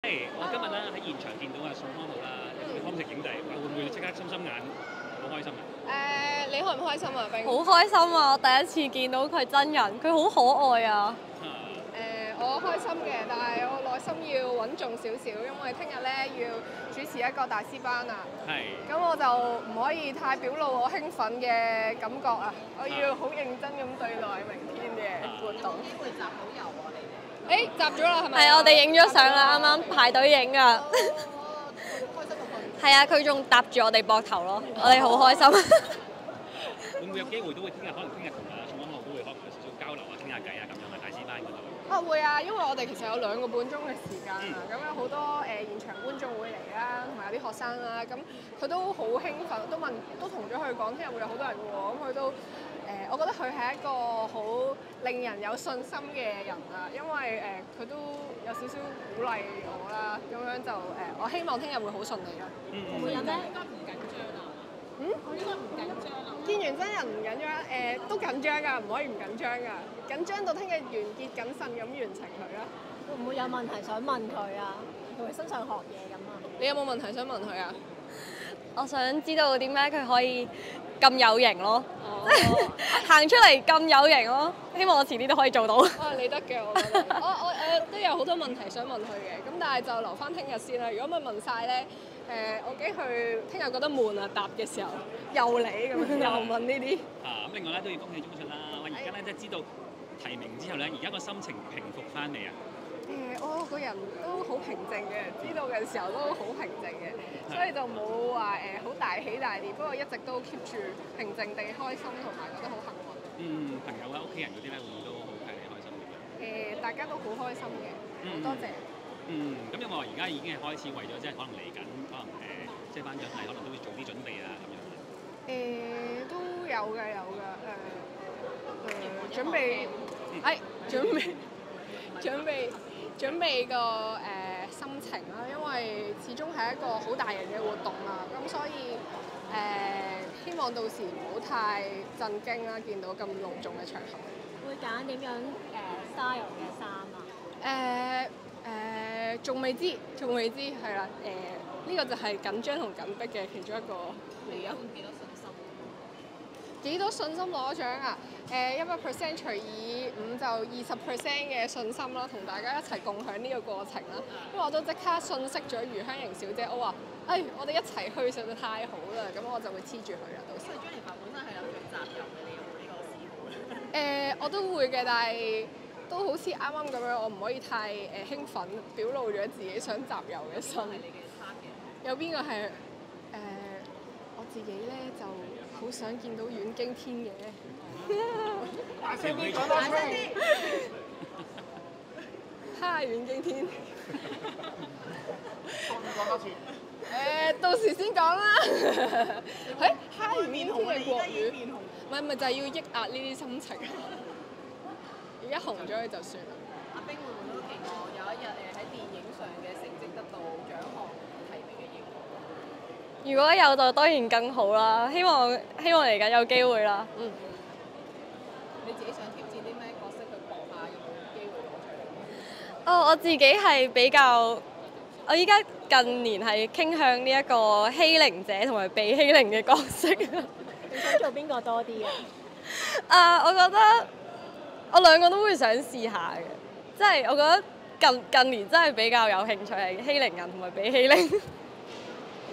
香港亞洲電影節｜吳冰鍾雪瑩跟宋康昊集郵超興奮：佢真人超可愛
吳冰和鍾雪瑩擔任第20屆香港亞洲電影大使，齊齊出席開幕典禮，接受傳媒訪問。兩人難得見到真身宋康昊，當然把握機會集郵，吳冰難掩喜悅之情，「佢仲搭住我哋膊頭，好開心。她又大讚，「佢真人超可愛！」鍾雪瑩則表示，要壓抑興奮，因為今天要主持宋康昊與林嘉欣做嘉賓的大師班，不能失儀，「他是讓人有信心的心，也有鼓勵我，希望順利。」

bmgproductionmusic